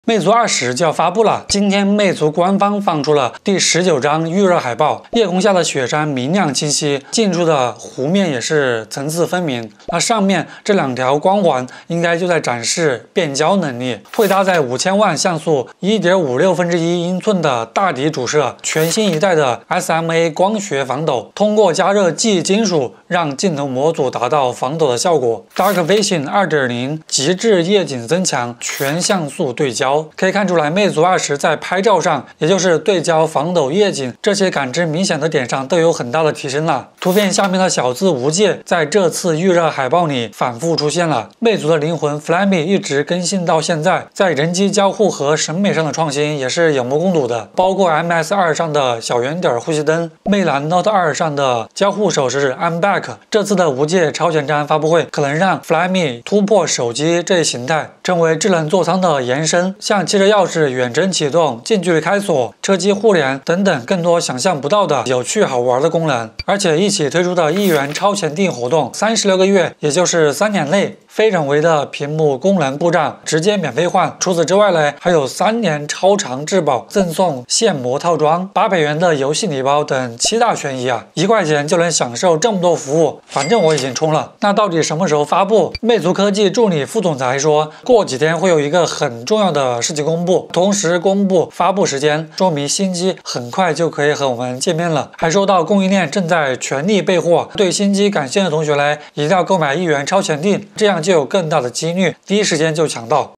【魅族20】📸 拿捏夜景拍摄，这次很有看头！ft.从手机卷到车机 | 科技小代
嗨喽，大家好，作为一个老魅友，聊聊魅族今天刚发布的夜景摄影功能，大光圈，防抖加变焦，夜景拍摄看来很不错！

【时间轴】
00:00 官宣海报
00:36 Flyme 车机延展
01:07 一元超前订
01:26 发布时间